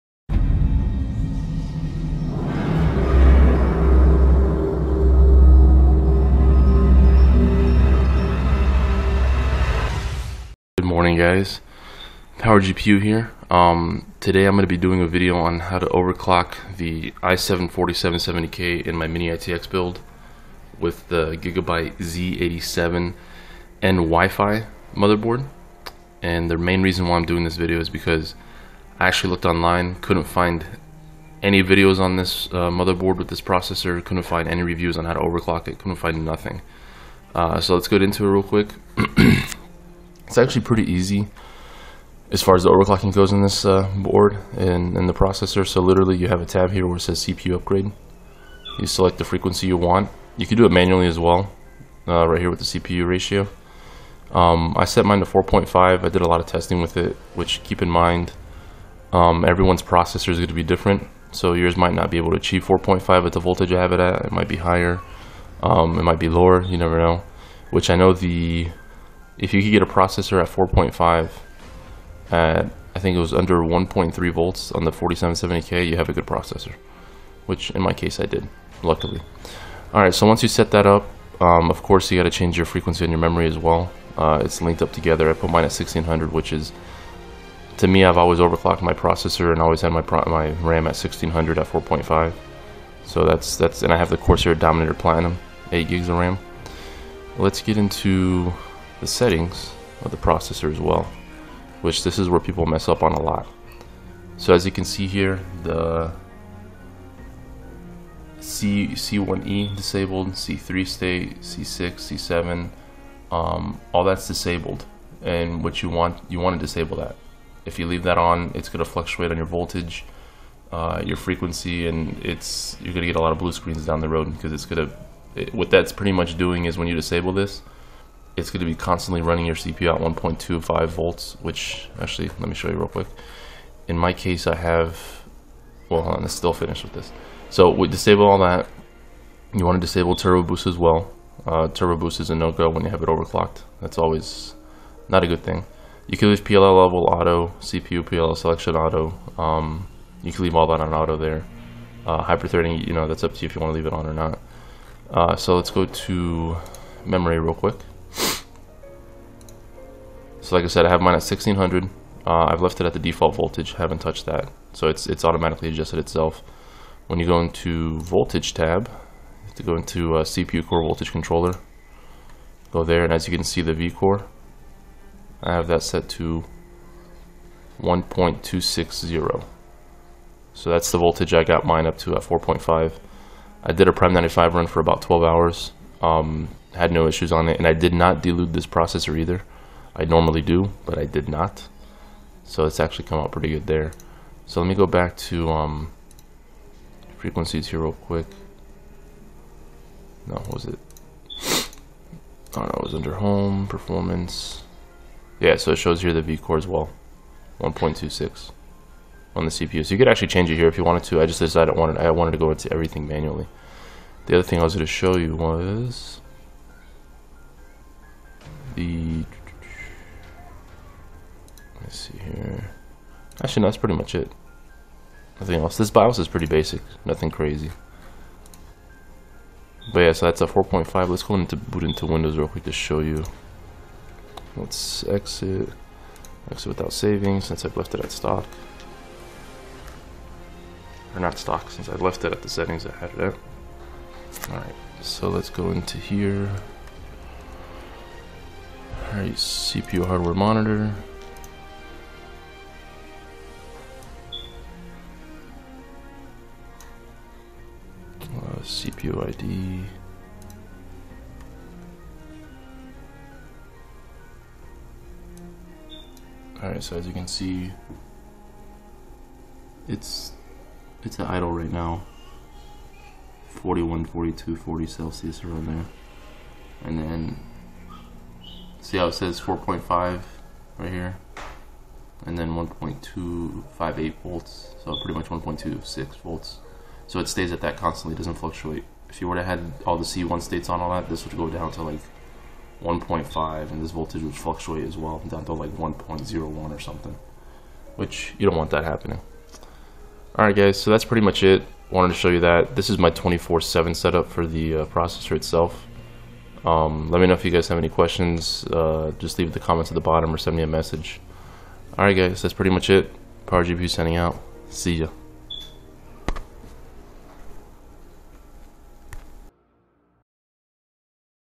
i7 4770k@4.5 Gigabyte z87n-wifi - How to overclock
Just wanted to show how to oc the i7 4770k with this motherboard since I couldn't find anything about it online myself